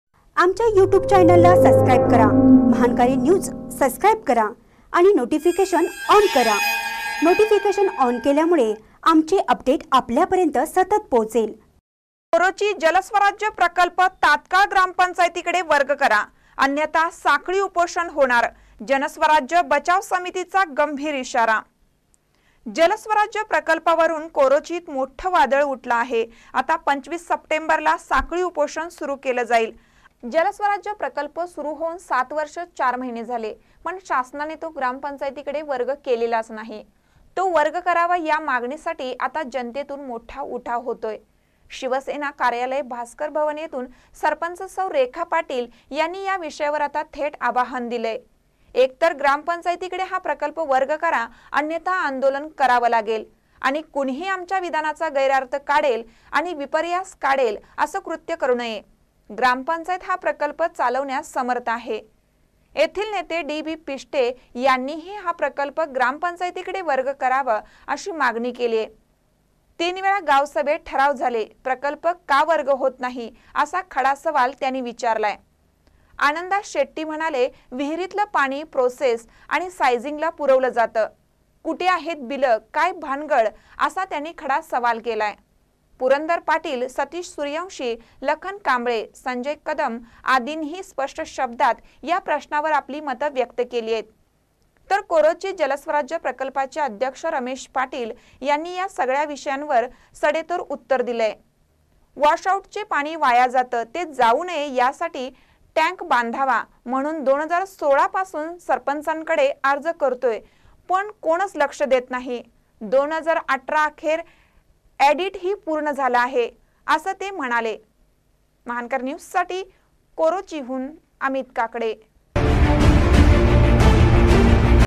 कोरोची जलस्वराज्य प्रकल्प तात्काळ ग्रामपंचायतीकडे वर्ग करा. अन्यथा साखळी
कोरोची जलस्वराज्य प्रकल्प तात्काळ ग्रामपंचायतीकडे वर्ग करा. अन्यथा साखळी उपोषण होणार. जनस्वराज बचाव समितीचा गंभीर इशारा.